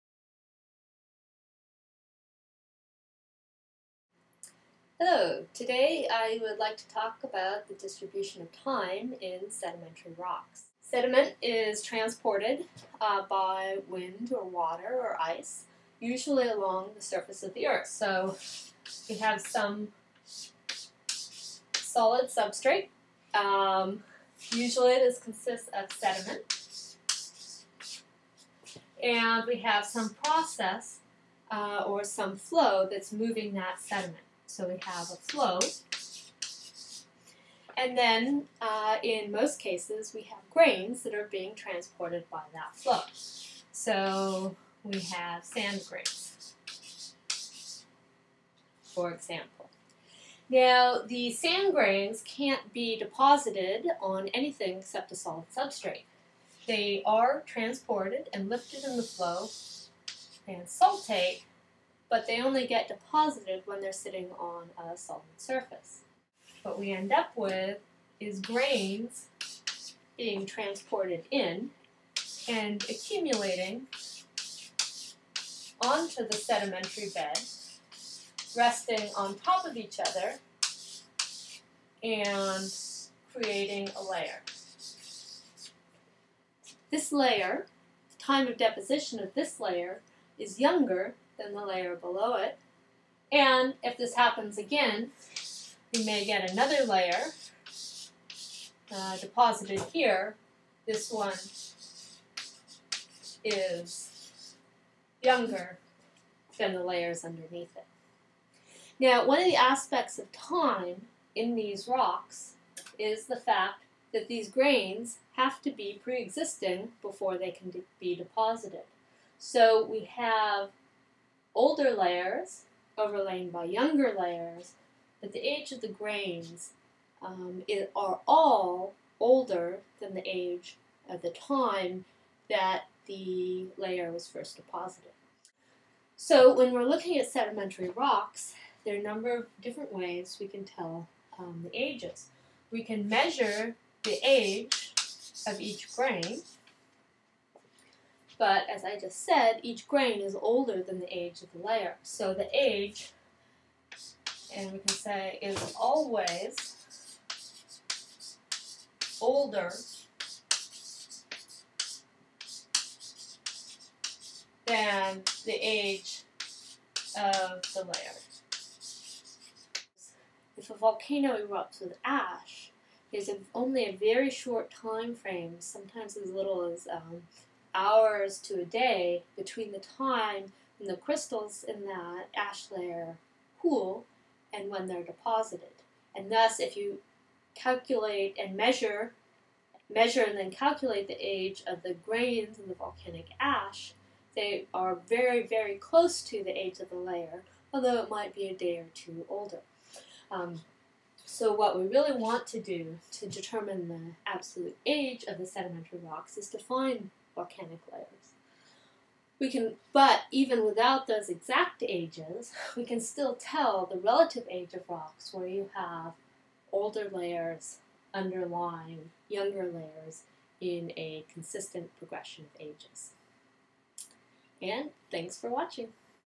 Time in Sedimentary Rocks (sed strat)
In this video, I discuss the idea that older layers underlie younger layers.  However, the age of individual grains can be significantly older than the age of the layer they were deposited in because the grains have to exist before they are deposited.  In the special case of layers of volcanic ash, the ages of crystals in the ash might be only slightly older than the age of the layer they are in, which provides excellent opportunities for measuring the absolute ages of sediments.